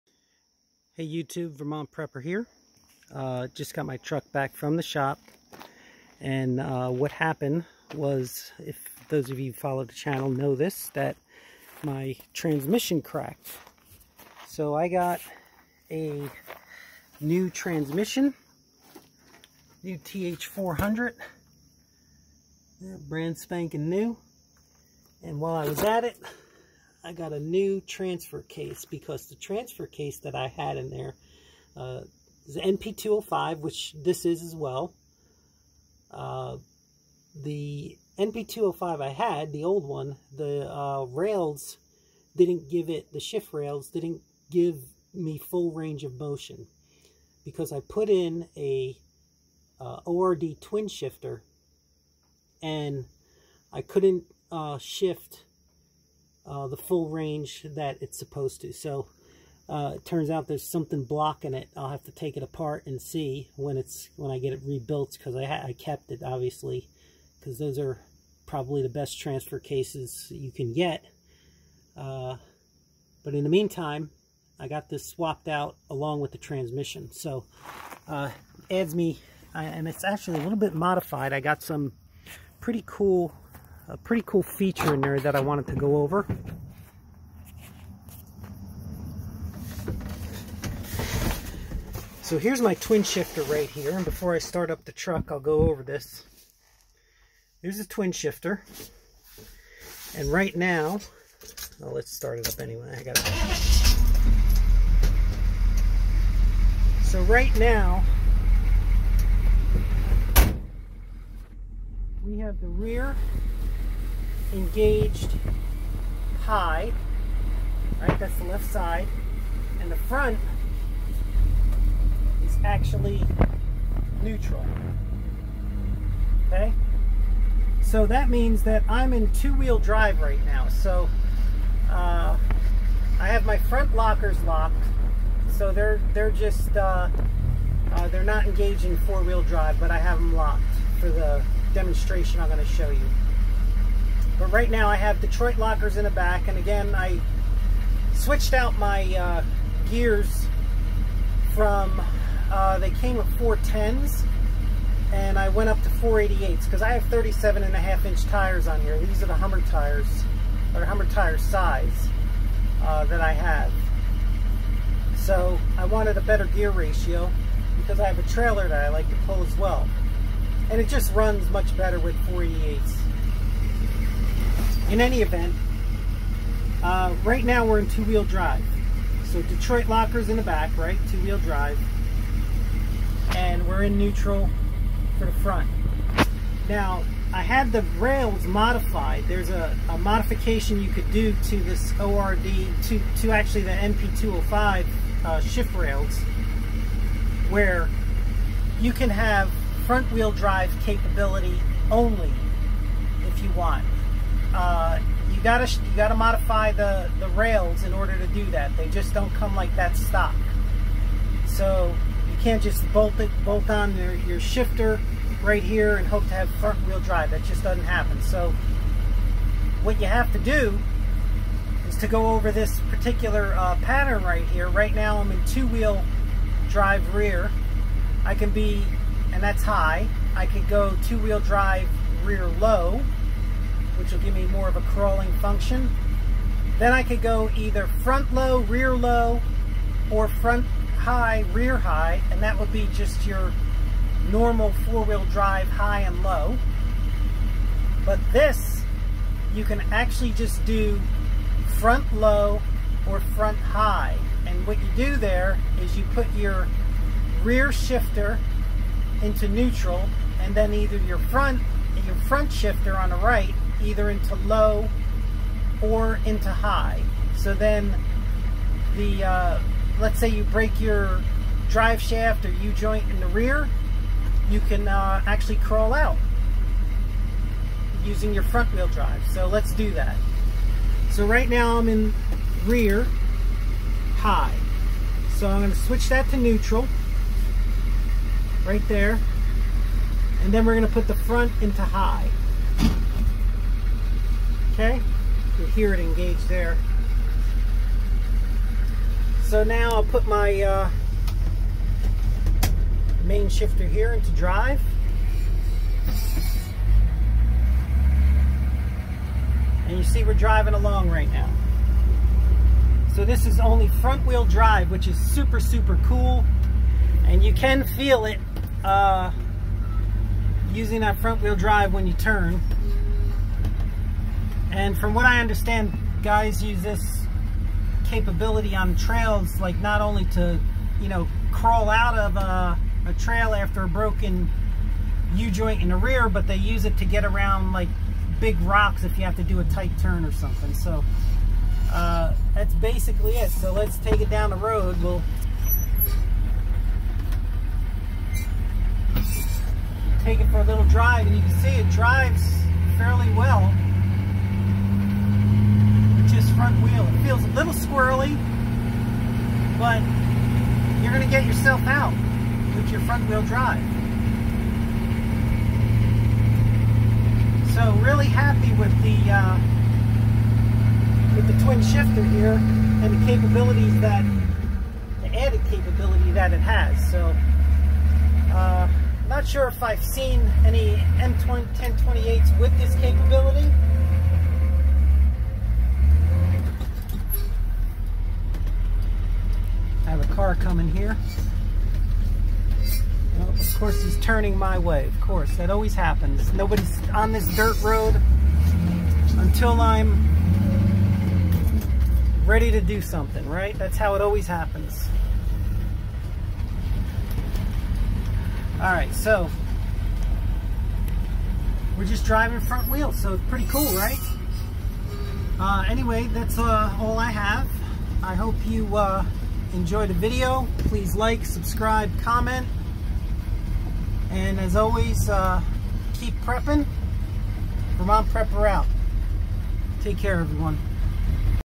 M1028A2 CUCV Project: - Demo! Front-Wheel Drive-Only w/ ORD Twin Shifters & Modified Rails
I just got my truck back from the shop after replacing both the TH400 transmission (old one cracked) and NP205 transfer case (old shift rails did not work properly).  I also had the shift rails modified in the new NP205 transfer case to give me a front wheel drive-only capability utilizing the ORD twin shift kit.  Its a super cool feature, especially if you break a rear shaft or U-Joint.  You can just remove the rear shaft and rescue yourself in front wheel drive.  This feature is only available if the rails are modified.  ORD sells the kits so you don't need to modify the rails yourself.  It will require that you partially take apart the transfer case and put in the modified rails.  This video demonstrates the front wheel drive capability.